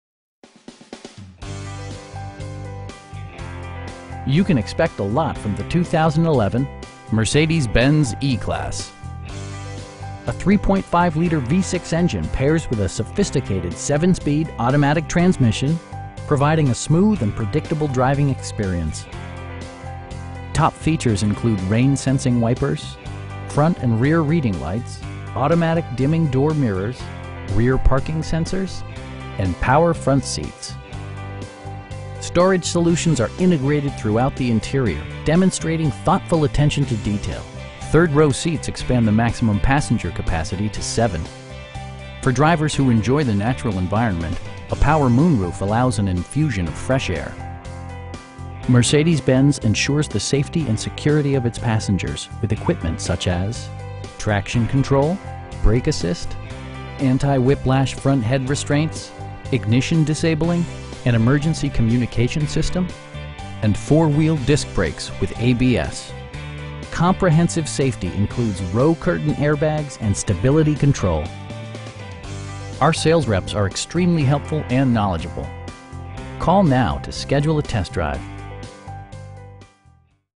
2011 Mercedes-Benz E-Class E350 4MATIC
Crown Eurocars
6001 34th St. N.

You can expect a lot from the 2011 Mercedes-Benz E-Class. A 3.5 liter V-6 engine pairs with a sophisticated 7 speed automatic transmission, providing a smooth and predictable driving experience. Top features include rain sensing wipers, front and rear reading lights, automatic dimming door mirrors, rear parking sensors, tilt and telescoping steering wheel, power windows, an overhead console, and power front seats. Storage solutions are integrated throughout the interior, demonstrating thoughtful attention to detail Third row seats expand the maximum passenger capacity to 7. For drivers who enjoy the natural environment, a power moon roof allows an infusion of fresh air. Mercedes-Benz ensures the safety and security of its passengers with equipment such as: traction control, brake assist, anti-whiplash front head restraints, ignition disabling, an emergency communication system, and 4 wheel disc brakes with ABS. Comprehensive safety includes row curtain airbags and stability control. Our sales reps are extremely helpful & knowledgeable. They'll work with you to find the right vehicle at a price you can afford.